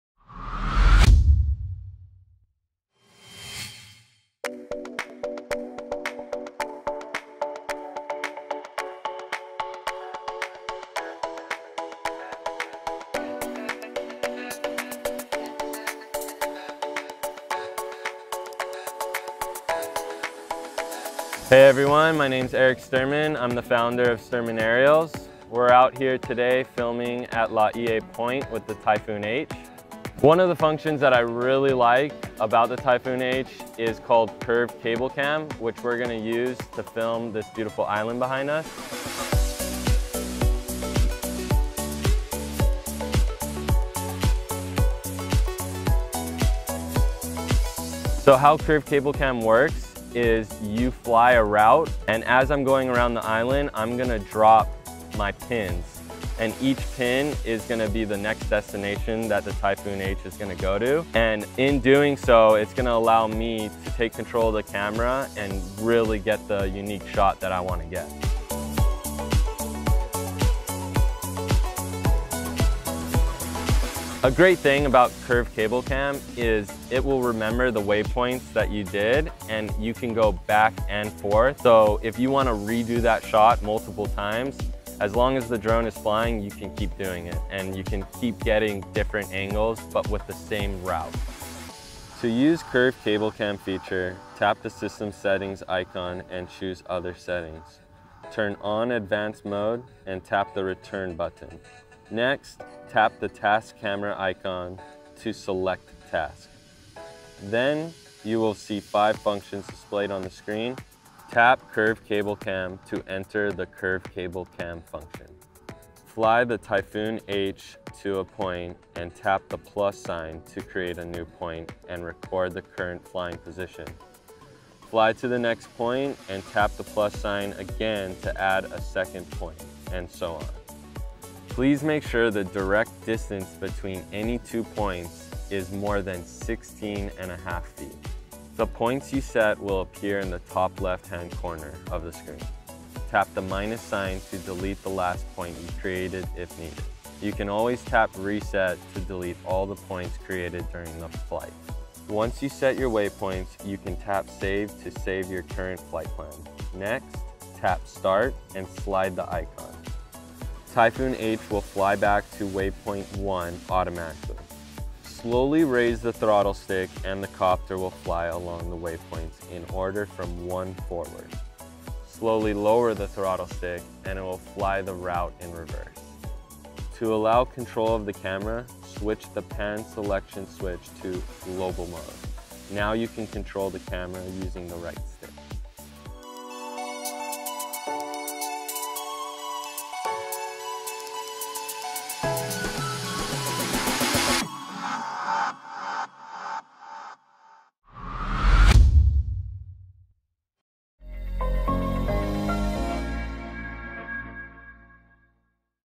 TYPHOON H - Curve Cable Cam (EN)
Eric Sterman of Sterman Aerials demonstrates Curve Cable Cam mode from a beautiful location in Hawaii. CCC mode allows the pilot to easily program an invisible route for Typhoon H to fly along. The pilot will fly between pre-set coordinates while independently controlling camera position.